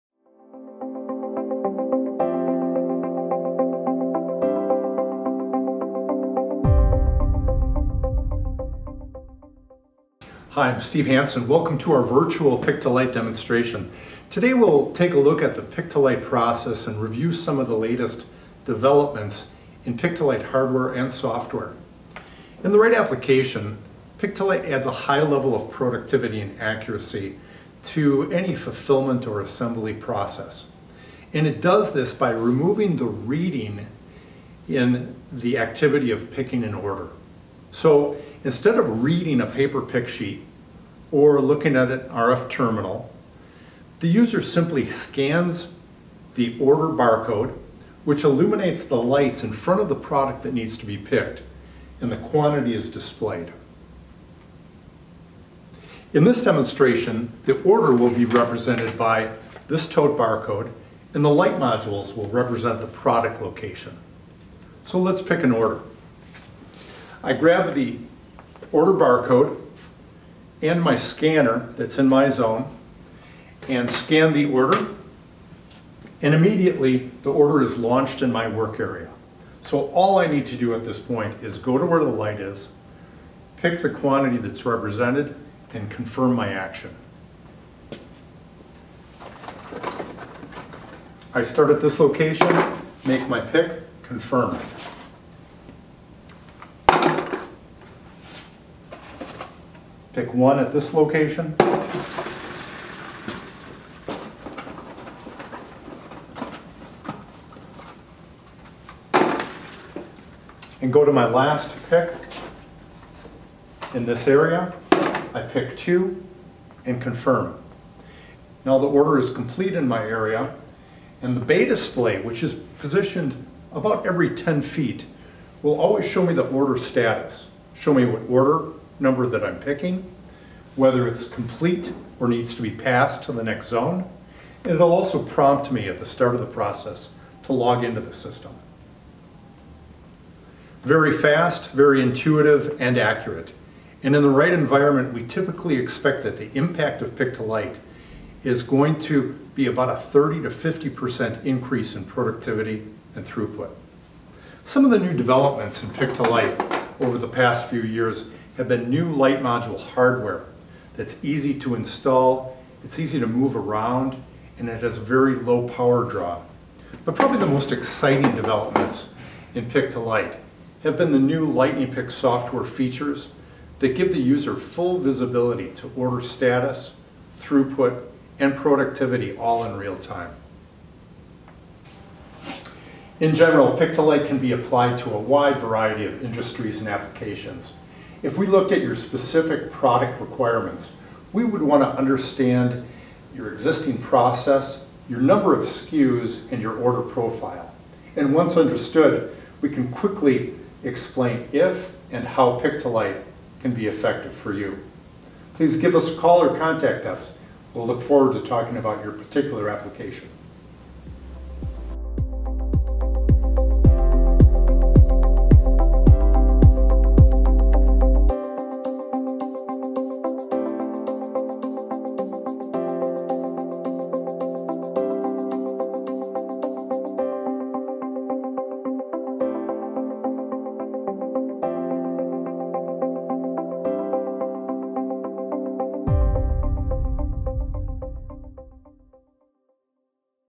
Pick to Light Demonstration
What is Pick-to-Light and how does it work? (Pick-to-Light is also known as Pick-by-Light.) This video shows a demonstration of light-directed order picking using the Lightning Pick system for fast and accurate warehouse order fulfillment. Pick-to-light is the operator-based picking strategy for high speed piece picking to execute broken case quantity order fulfillment. Ideal for team-based approaches like zone picking, Lightning Pick is the fastest growing, light-directed picking user community for 25 years.